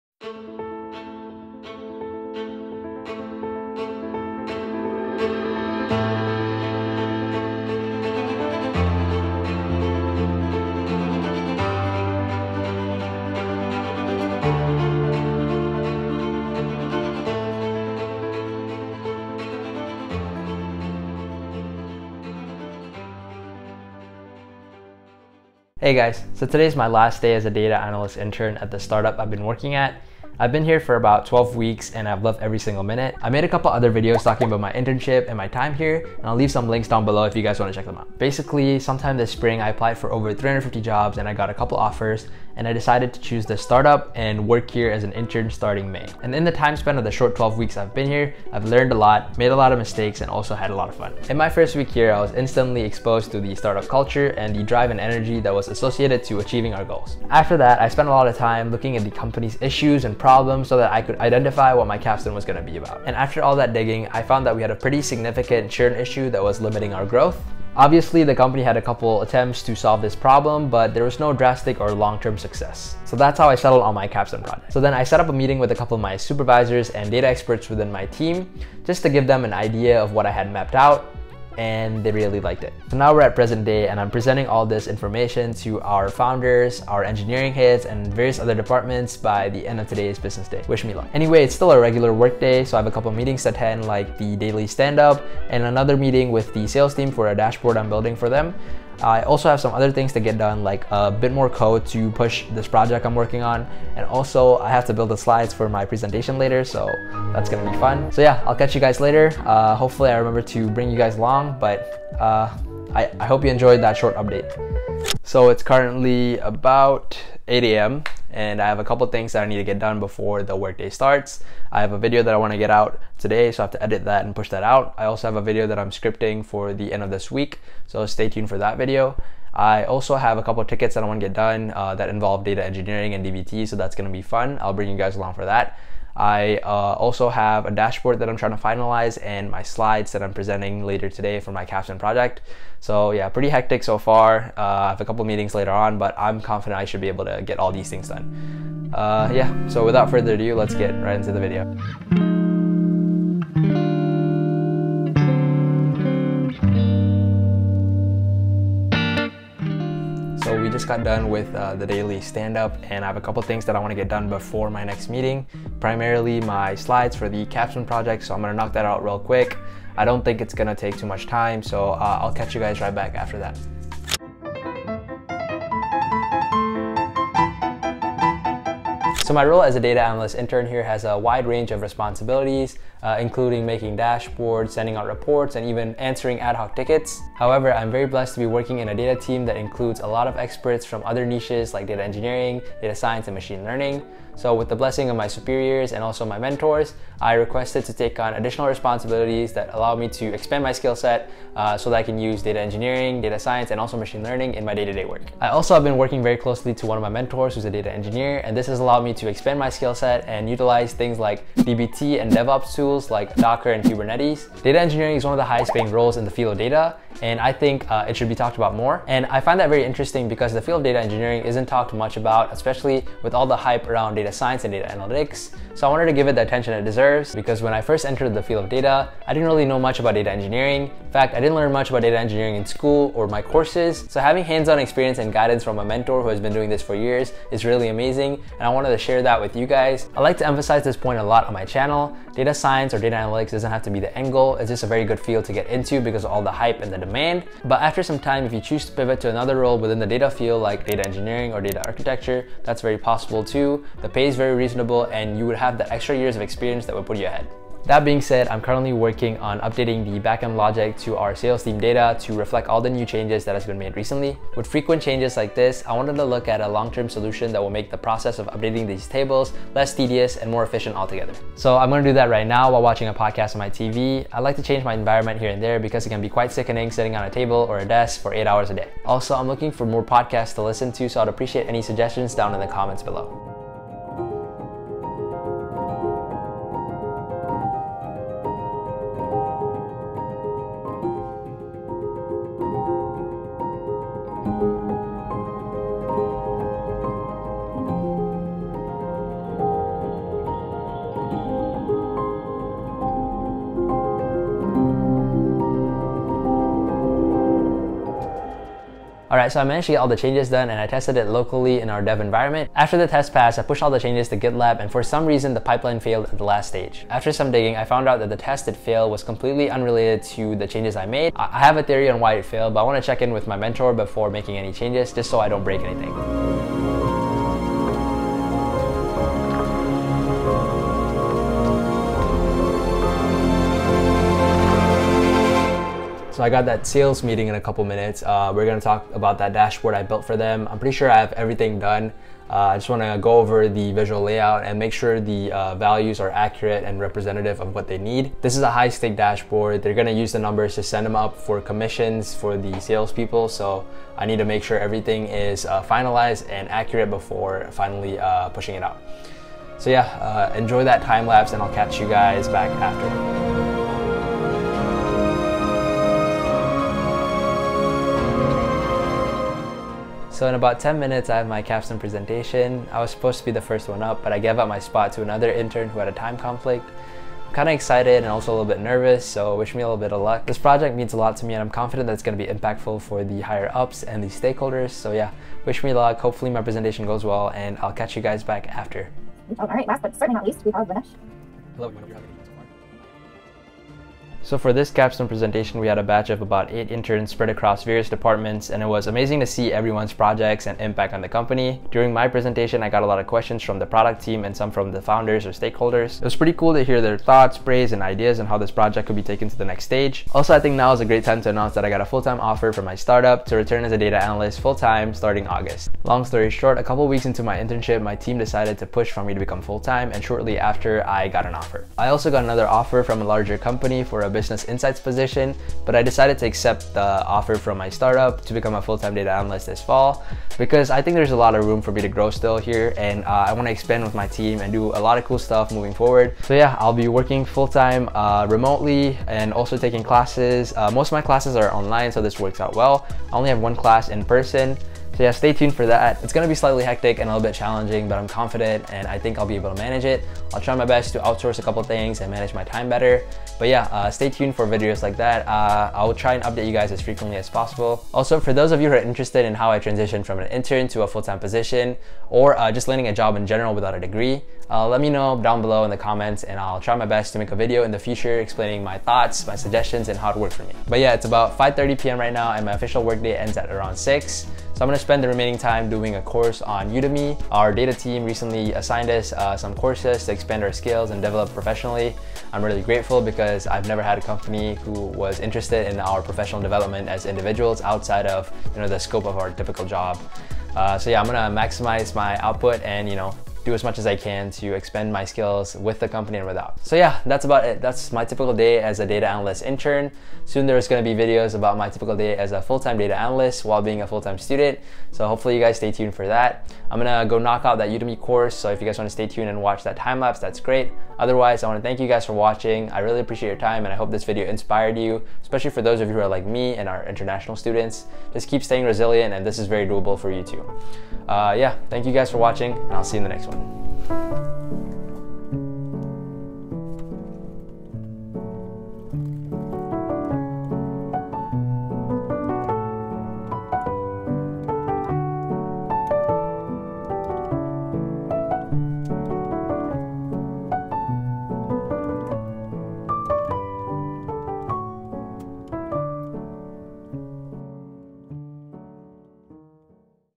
Day In The Life of a Data Analyst Intern at a Startup // Capstone Presentation
Today is my last day as a Data Analyst Intern at the startup I have been working at. We go through my routine as an intern and my capstone presentation for the project I have been working on this past 10 weeks.

5 Things I learned from my first week at my internship

Timestamps
0:00 Introduction
0:14 Quick Recap
0:35 Backstory
1:00 1st Week
1:04 Capstone Ideas
1:17 Capstone Proposal
1:26 Present Day
1:32 Breakdown of agenda
1:57 Morning routine
2:31 Daily Standup/Checkin
2:45 Slides Prep
2:55 Lunch Break
3:03 My Role
3:20 How I expand my skillset
4:00 How to pivot into Data Engineering
4:20 Data Analytics Engineering to-do
5:14 Roadblock in pipeline
5:38 Sales Team Dashboard
6:28 Capstone Presentation
6:50 My Big News!
7:16 What's Next?
7:39 How to land a data job
7:54 Evening routine
8:12 Closing Remarks
8:20 Bonus Clip